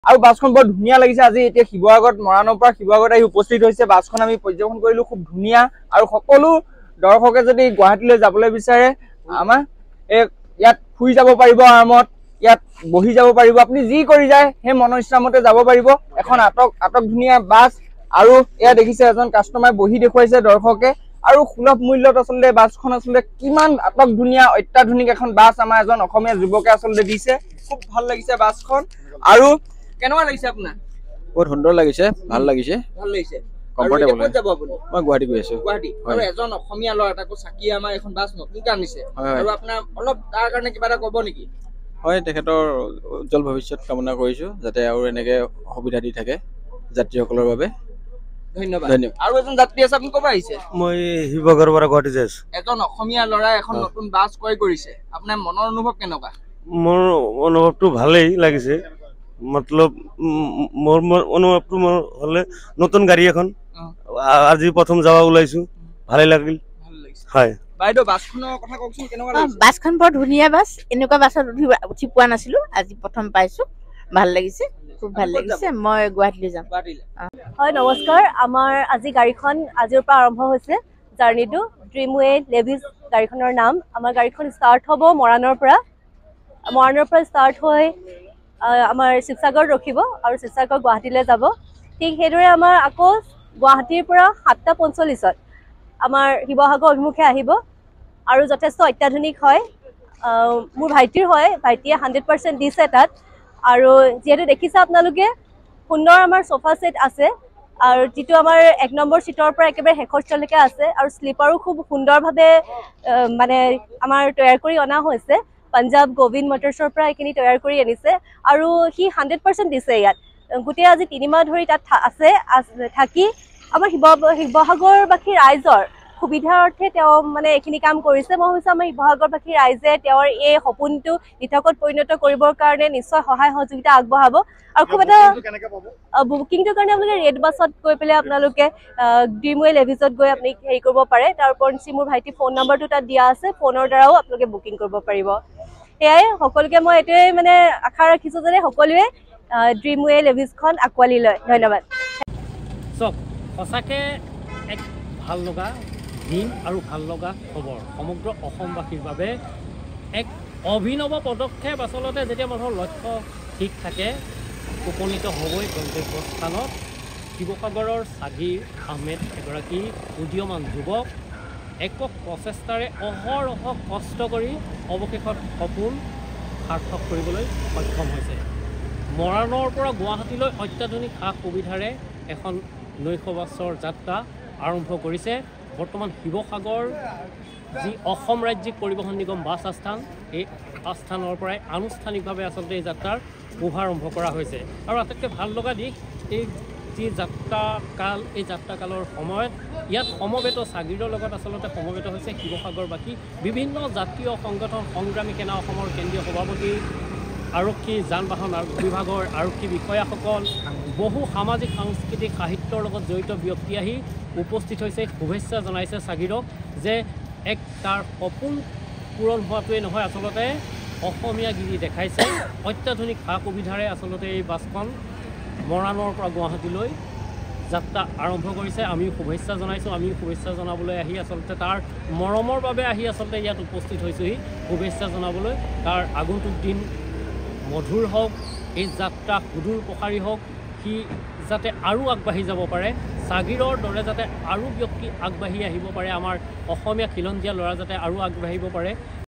Dreamway lavish.
A luxury bus starting from Moran to Guwahati..